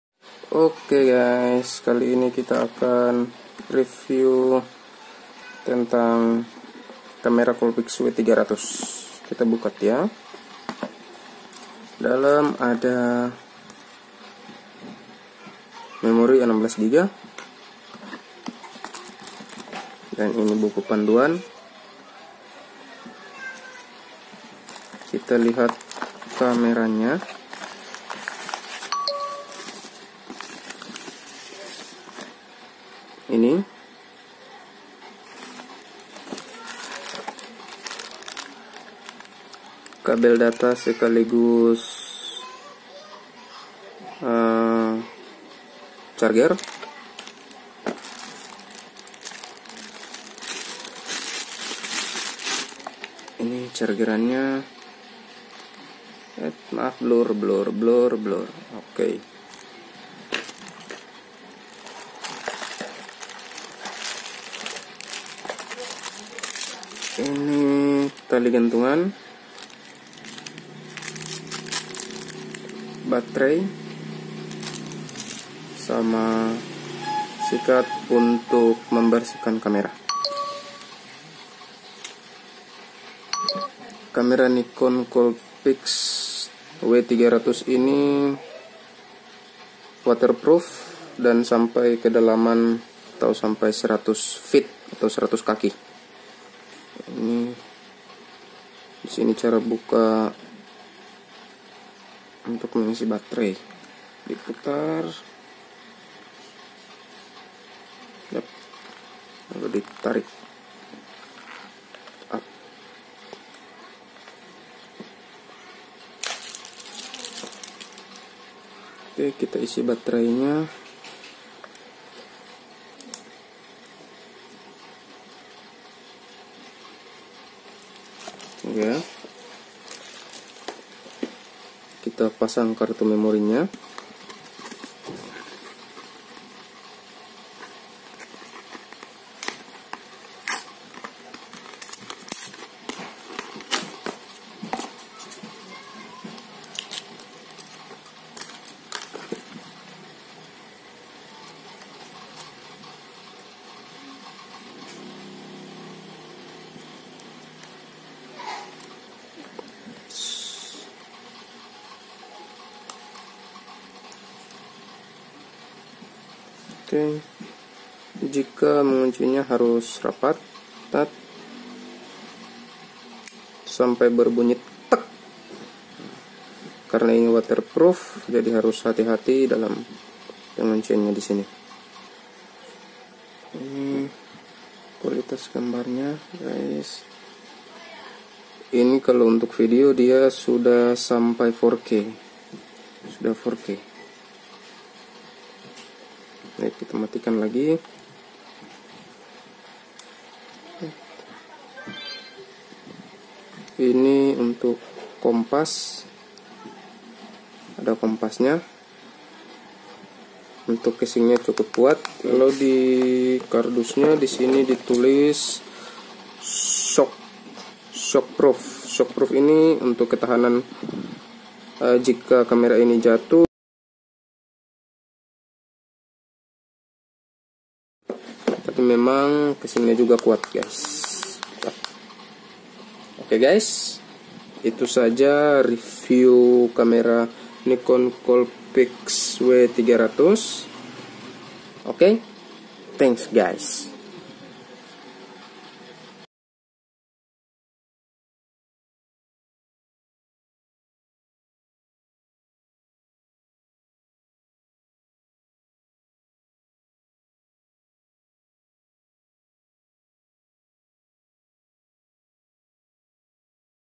UNBOXING dan REVIEW KAMERA NIKON COOLPIX W300
Kamera Nikon Coolpix w300 adalah kamera waterproof yang tahan hingga kedalaman 30m atau 100ft. Kamera ini juga dilengkapi kompas, GPS dan senter. Hasil videonya sudah 4K atau ultra HD, jadi cocok buat merekam video.